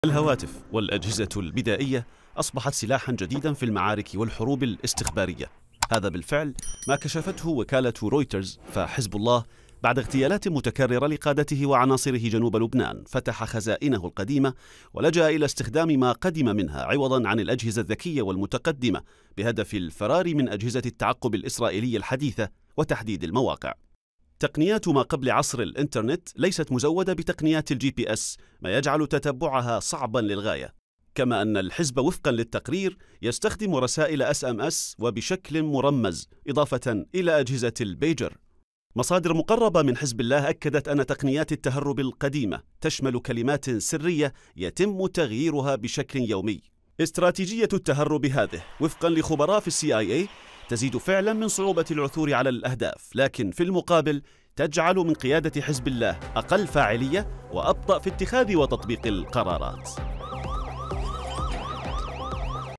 للتهرب من المراقبة الإسرائيلية.. "حزب الله" يستخدم أجهزة اتصال بدائية
كشف تقرير لوكالة "رويترز" أن "حزب الله" لجأ إلى استخدام الهواتف القديمة بدلا من الأجهزة المتقدمة للتهرب من الاستخبارات الإسرائيلية بعد اغتيال عدد من قيادته وعناصره جنوب لبنان ما يجعل تتبع هذه الأجهزة صعب الوصول إليها.